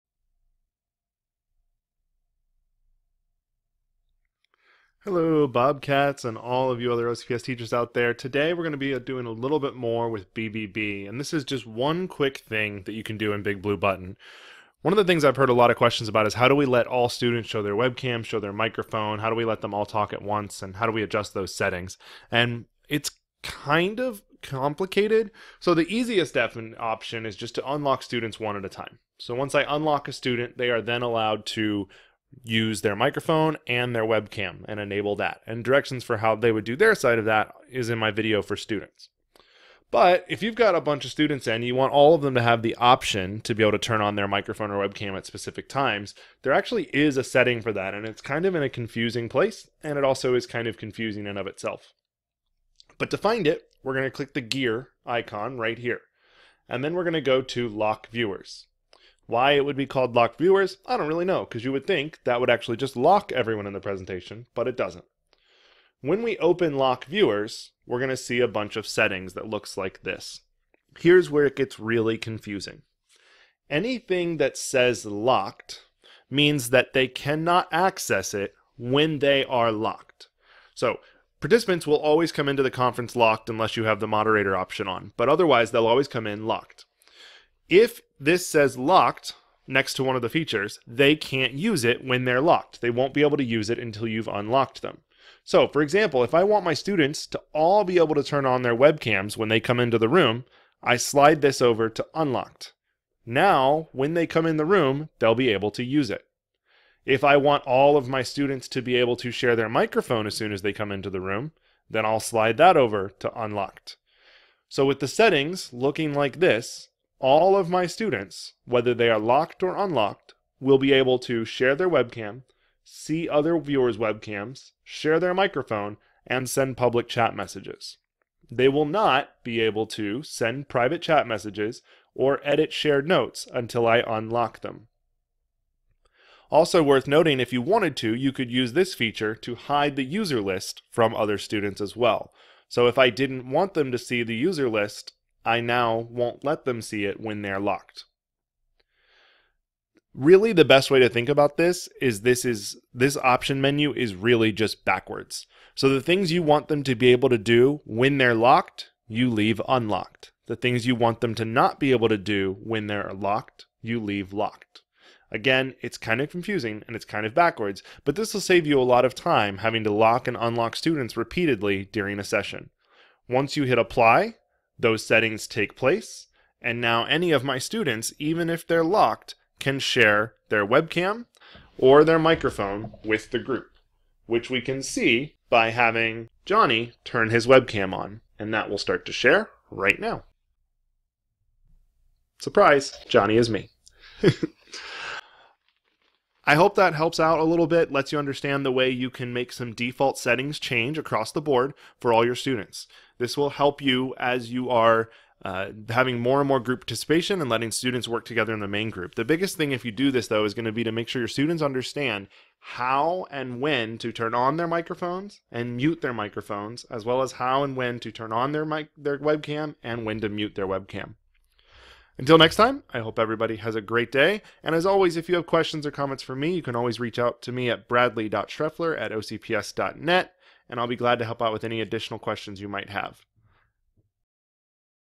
BBB - See Student Webcams without Unlocking Each Student
This video will show you a trick to setting the default sharing capabilities of participants in your BigBlueButton session.